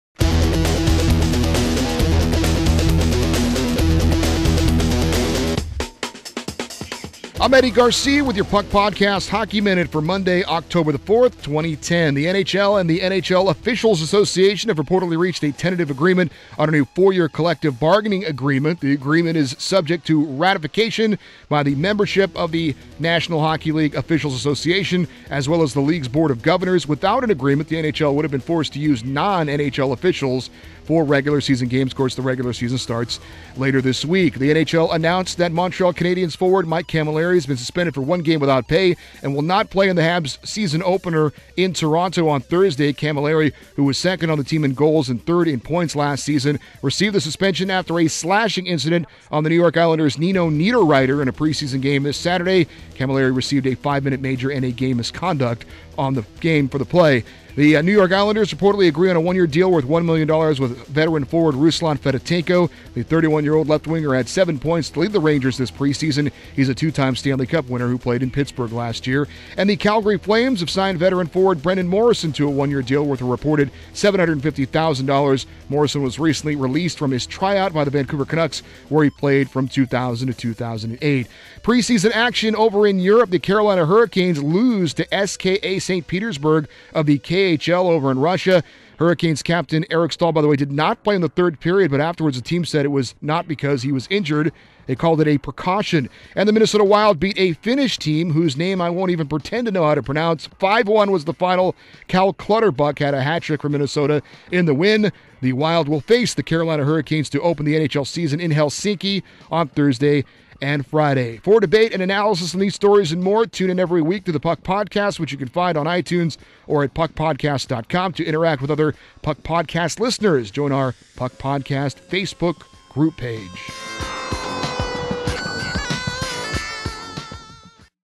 Puck Podcast Hockey Minute 10-4-10
Get news on a top NHL player getting suspended and a couple of NHL players signing new deals and more.